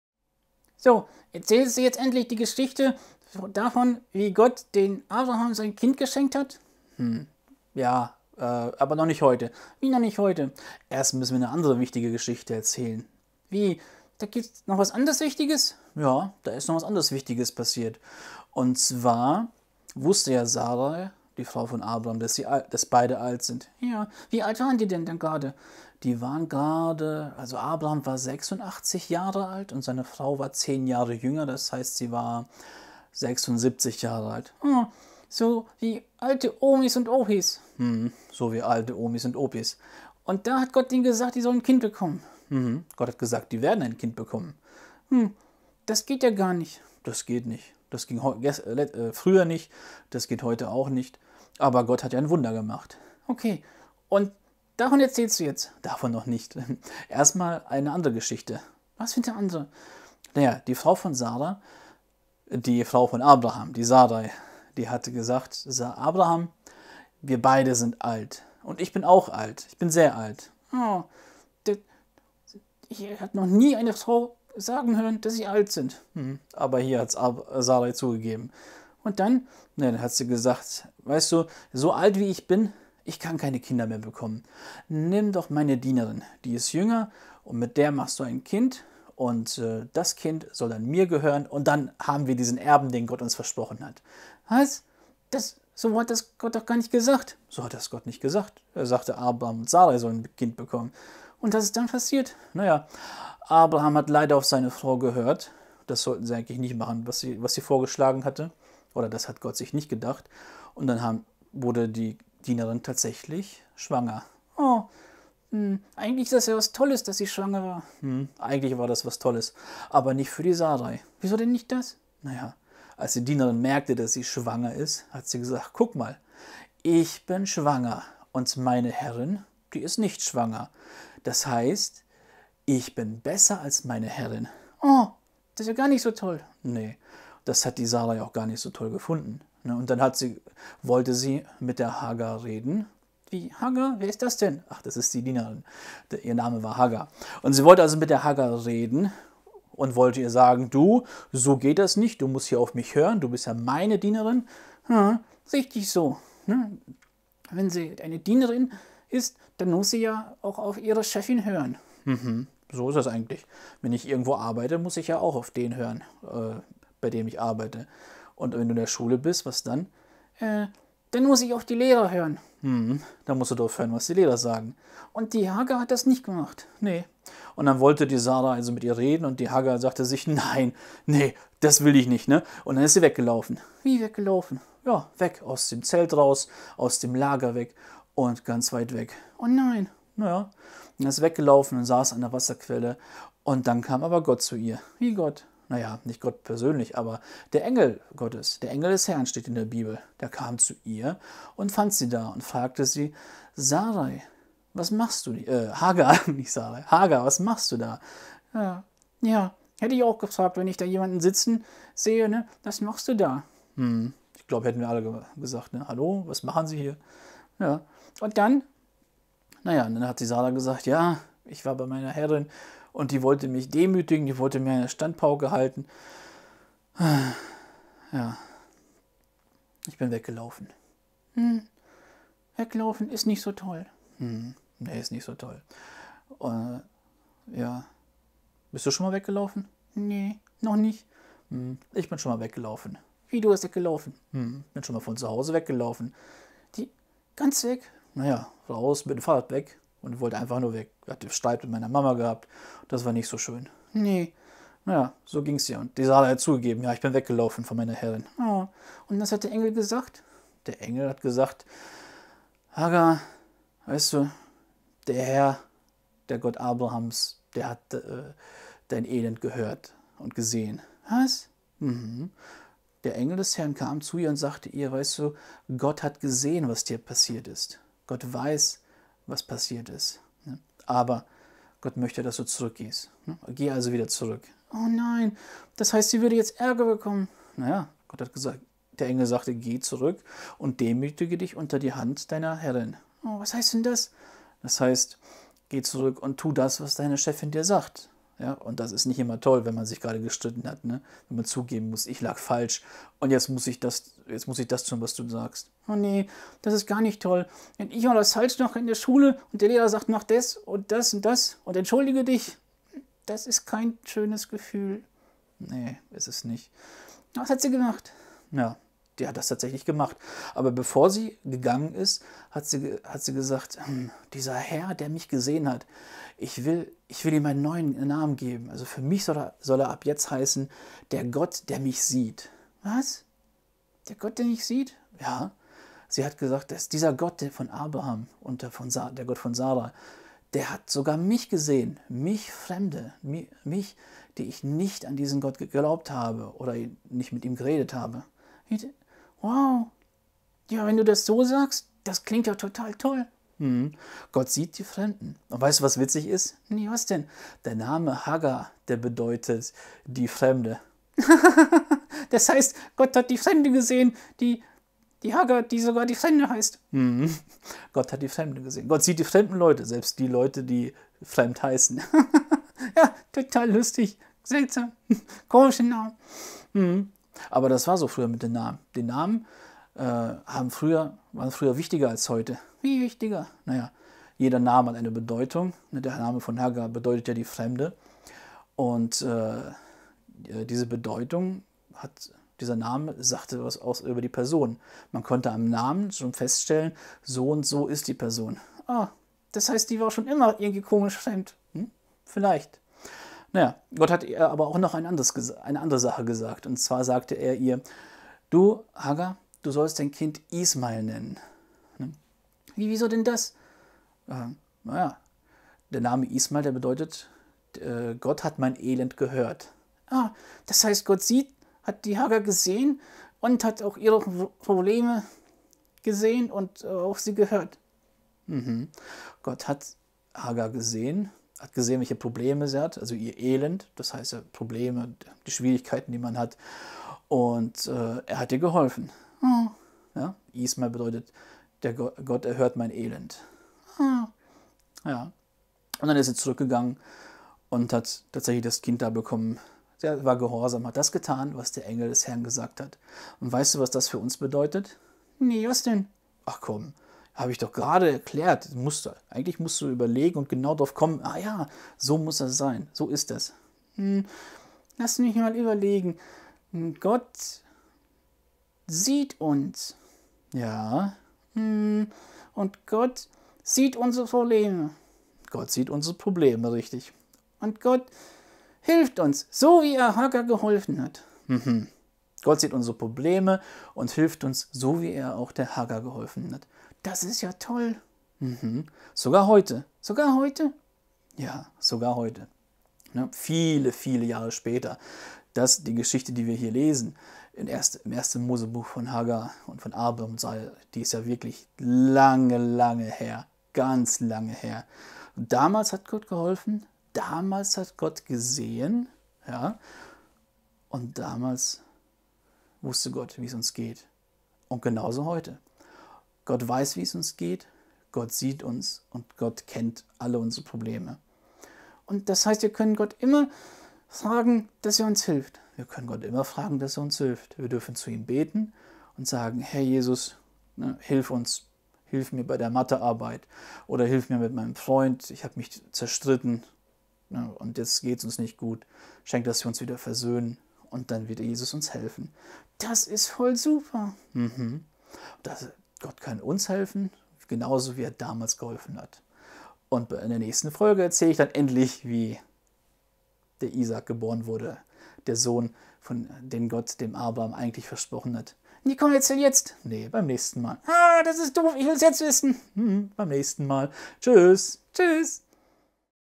Jack und Eddy - Folge 12 - Der Gott der sieht
Hagar erlebt, dass der Gott Abrahams auch sie sieht, und sich um sie kümmert.

Wenn du Fragen hast, schreib mir doch:
jack @ andrejev . de (ohne Leerzeichen)